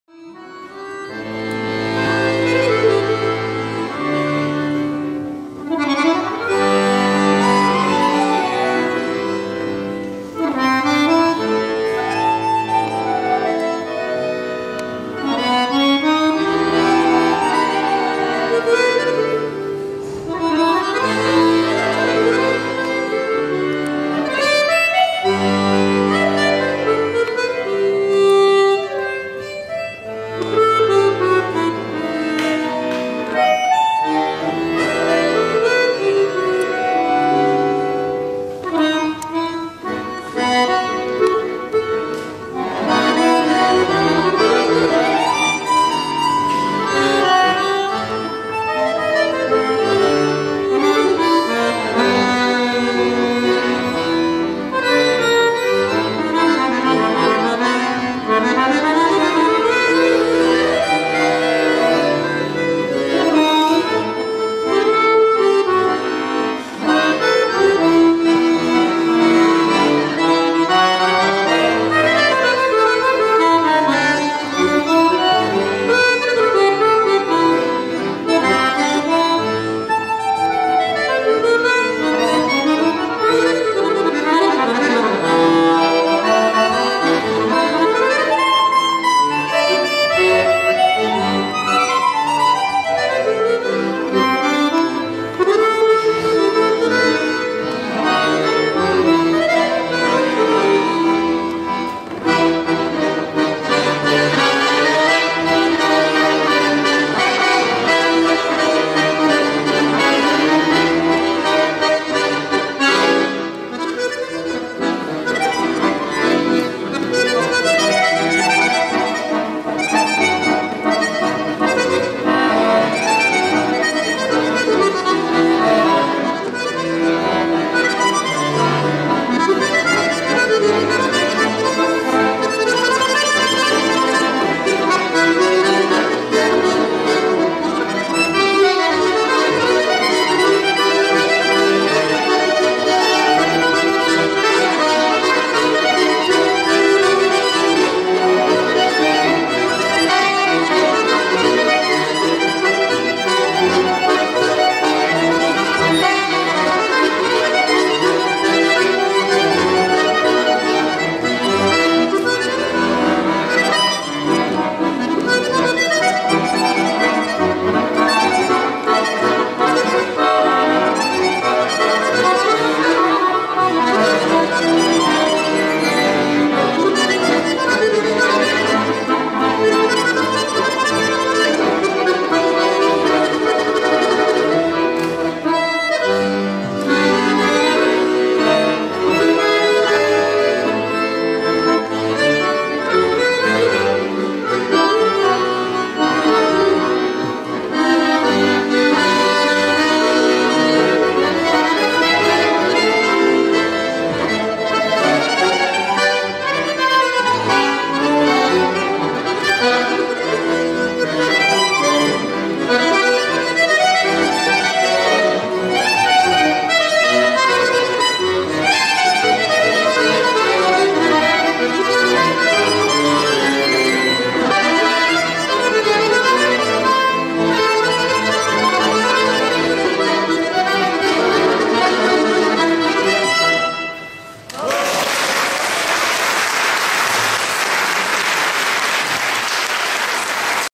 Nicolas LEBRET Camille PRIVAT Raulhac aout 2018 Duo accordéon Pot pourri
Festival de l'Accordéon 18e édition - RAULHAC (15) du 2 au 5 août 2018 - Concert DUO Camille PRIVAT et Nicolas LEBRET accordéon - Vidéo et réalisation Alexandra Astorg (Sandy)